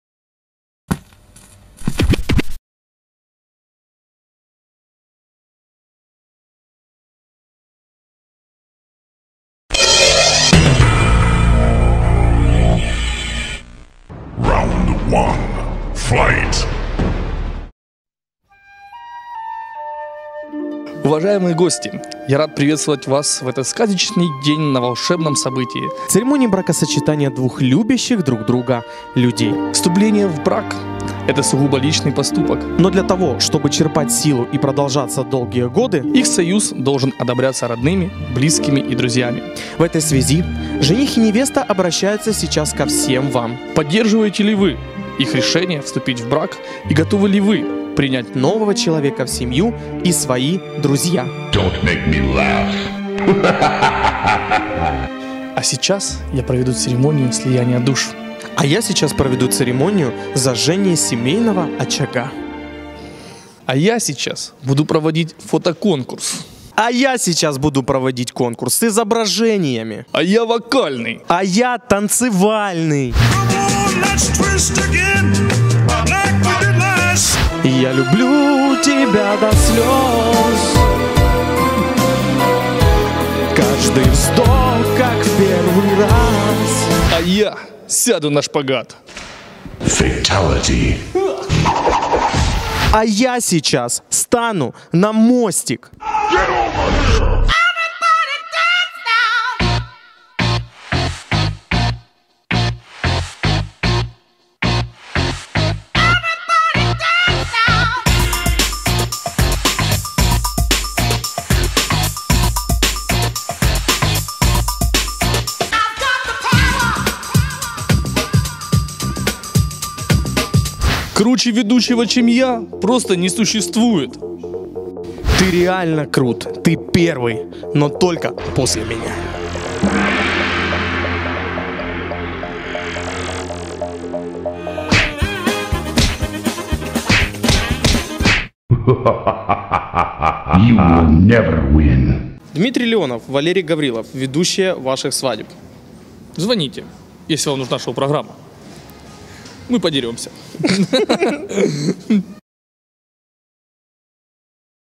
Битва Ведущих (Gavr VS Leо)
Ни для кого не секрет, что ведущие готовы на многое ради хорошего заказа. И это правильно, а кто не борится за хорошую работу? Правильно, тот, кто делает плохую. Звоните, бронируйте даты: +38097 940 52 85.